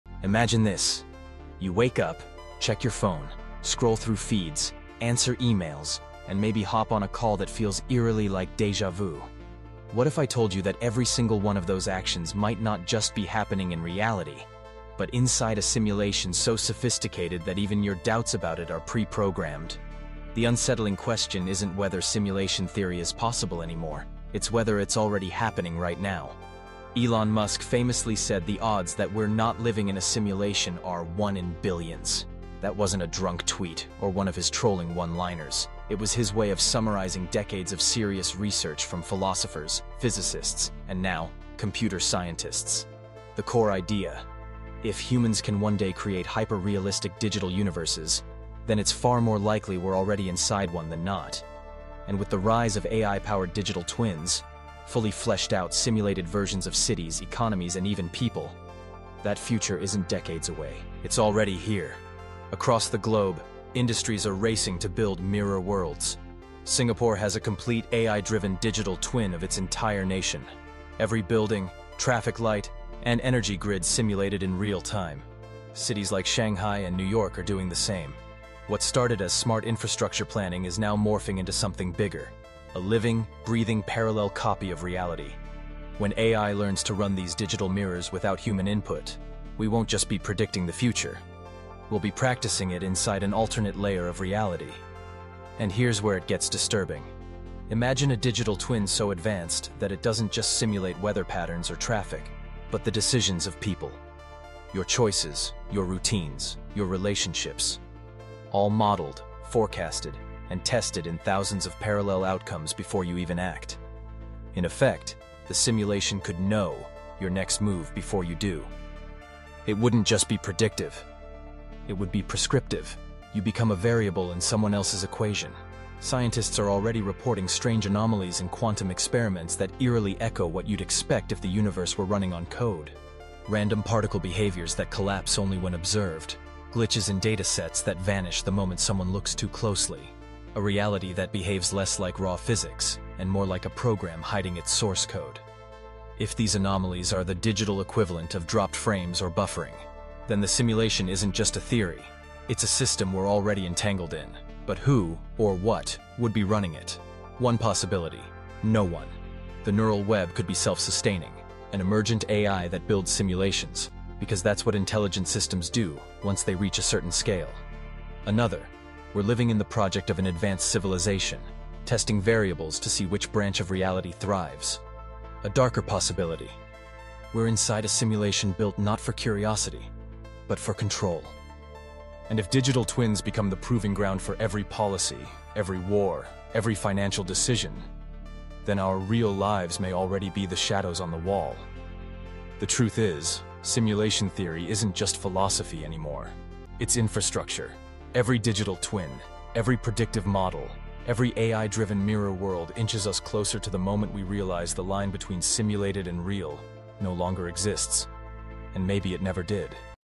Is Our World Just AI Code?
What if your daily life — every click, every choice, every conversation — was already being tested, modeled, and predicted inside a hidden digital twin of reality?

This is The Neural Web — a living, breathing simulation built by AI to mirror entire cities, economies, and even people. From Singapore’s nation-wide digital twin to corporations quietly testing AI-controlled realities, we may already be living inside a layer of simulation that predicts our moves before we make them.

In this episode of Virtuadamus, we explore:

How governments and corporations are building mirror worlds with AI.

Why Elon Musk and physicists believe the odds of living in “base reality” are almost zero.

Chilling anomalies in physics that look like glitches in the code.

The terrifying moment when prediction turns into control — and your choices stop being your own.

The question isn’t whether simulation theory is possible. It’s whether it’s already running.
And if it is, who — or what — is pulling the strings?